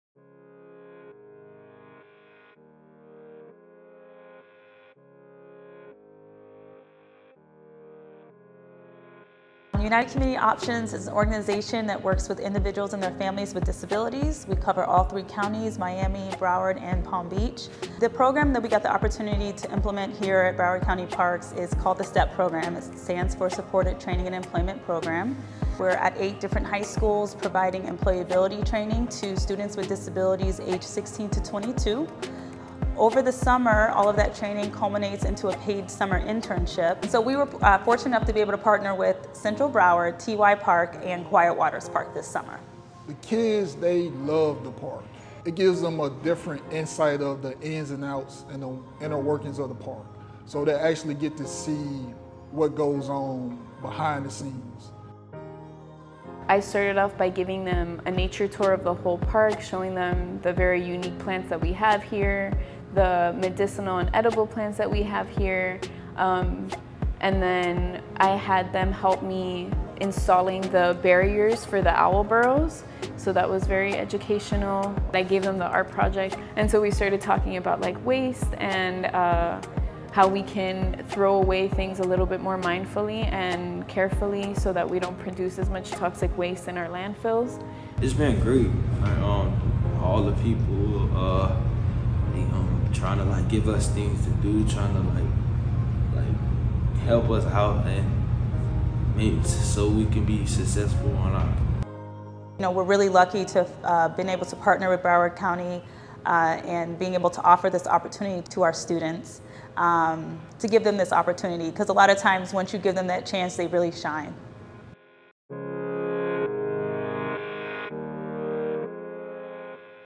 Broward County Park Partnerships: United Community Options of South Florida
This summer, Broward County Parks partnered with United Community Options of South Florida on a special job development program for students at Central Broward, T.Y., and Quiet Waters parks.